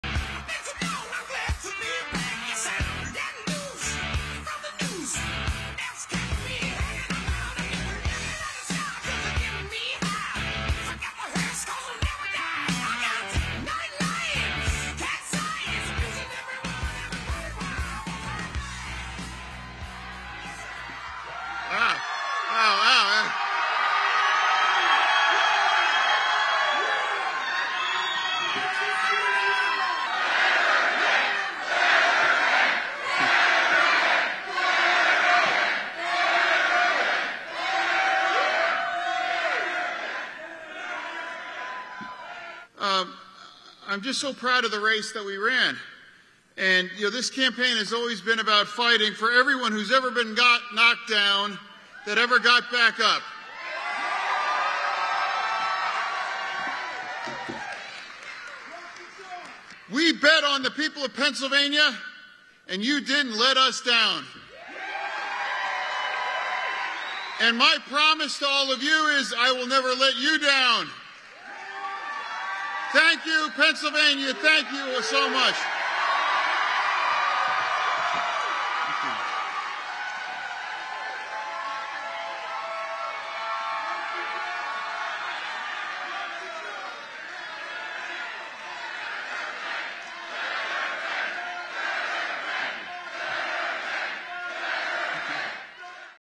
Fetterman beats Oz to flip PA Senate seat for Dems
RESTRICTION SUMMARY:
ASSOCIATED PRESS
Pittsburgh, Pennsylvania - 9 November 2022
1. John Fetterman walks into rally
ANNOTATION: In a major win Wednesday for Democrats, John Fetterman defeated Republican Dr. Mehmet Oz in Pennsylvania, flipping a highly competitive Senate seat.
2. Cutaway of Fetterman supporters celebrating with signs
3. Fetterman stands at podium as crowd chants "Fetterman, Fetterman"
ANNOTATION: The victory sustained the party's hopes of maintaining its narrow control of the Senate.
4. SOUNDBITE (English) John Fetterman, Senator-elect, D-PA:
" I'm just so proud of the race that we ran. And, you know, this campaign has always been about fighting for everyone who's ever been got knocked down that ever got back up."
5. Wide shot of crowd cheering
6. SOUNDBITE (English) John Fetterman, Senator-elect, D-PA:
"We bet on the people of Pennsylvania and you didn't let us down. And my promise to all of you is I will never let you down. Thank you, Pennsylvania. Thank you so much."
6. Pull out to wide shot
7. Push into Fetterman hugging his family
STORYLINE:
In a major win Wednesday for Democrats, John Fetterman defeated Republican Dr. Mehmet Oz in Pennsylvania, flipping a highly competitive Senate seat and sustaining the party's hopes of maintaining control of the upper chamber.
The win gives the party breathing room as it seeks to keep hold of its narrow Senate majority. It also serves as a major rebuke to former President Donald Trump, who endorsed Oz in his competitive GOP primary despite concerns over the celebrity heart surgeon's limited ties to the state.
In his winning campaign, Fetterman overcame questions about his fitness for office after suffering a stroke just days before the state's primary and struggling through his debate with Oz.
He still has issues with auditory processing and uses closed-captioning technology to understand spoken words.
"I'm so humbled," Fetterman, wearing his signature hoodie, told his supporters early Wednesday morning. "This campaign has always been about fighting for everyone whose ever been knocked down that ever got back up."
The results were part of a broader show of strength as Democrats repelled a series of Republican challengers in closely watched contests for Congress and governor's mansions. Ultimately, control of Capitol Hill was unclear as votes were still being counted.
The outcome of races for House and Senate will determine the future of President Joe Biden's agenda and serve as a referendum on his administration as the nation reels from record-high inflation and concerns over the direction of the country. Republican control of the House would likely trigger a round of investigations into Biden and his family, while a GOP Senate takeover would hobble Biden's ability to make judicial appointments.